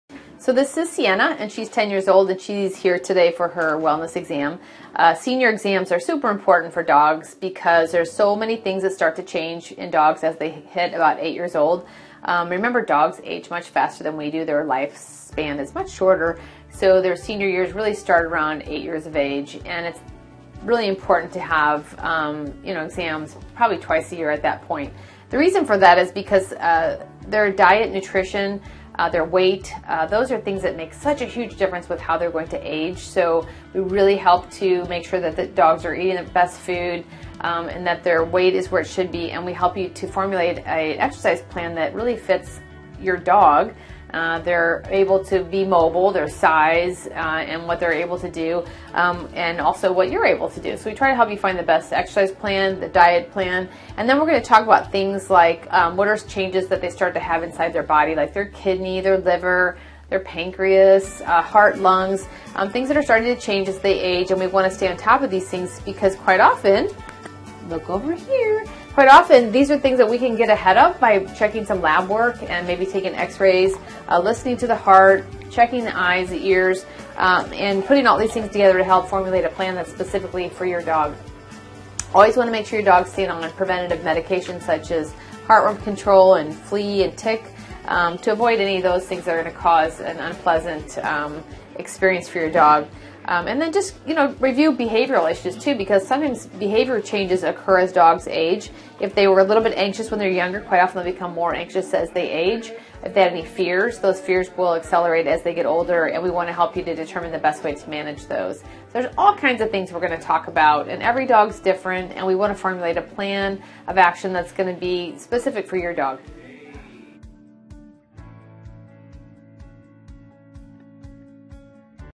Senior Wellness for Dogs
What you should consider during your dog's senior years